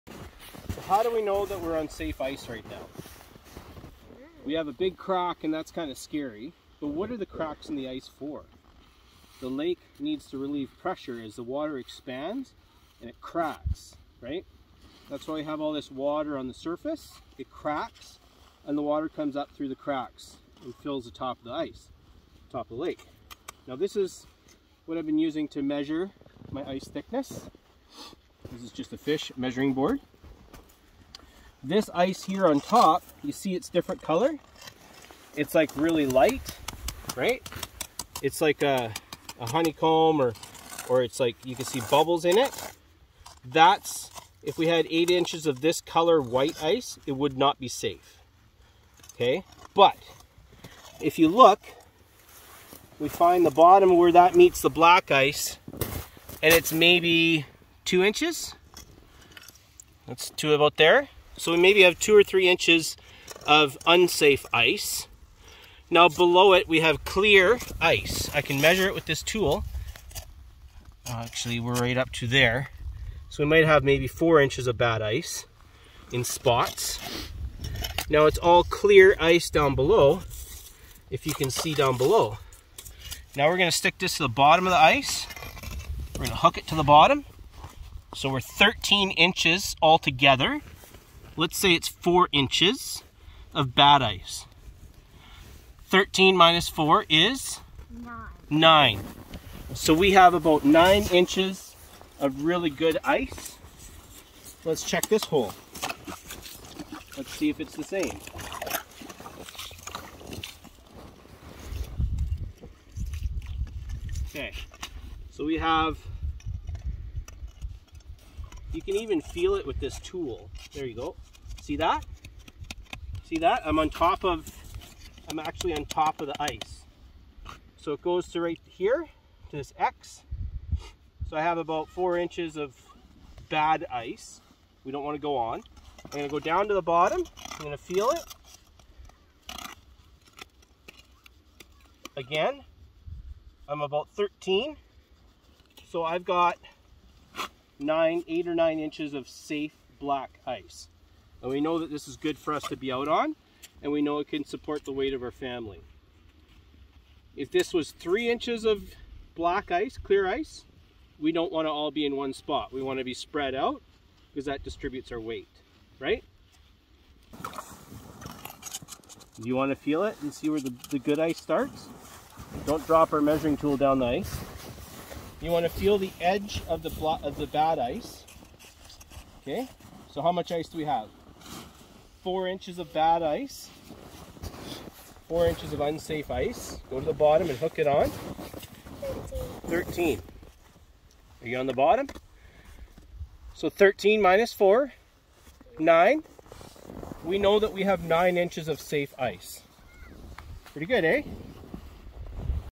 What is Safe Ice for Ice Fishing and How to Measure Safe Ice
In this video, the boys were nervous about going ice fishing, because there was water on top of the ice. They were not sure how thick the ice was on the lake. For assurance, we took a few minutes to measure and learn about what the different types of ice look like and what is safe ice fishing thickness.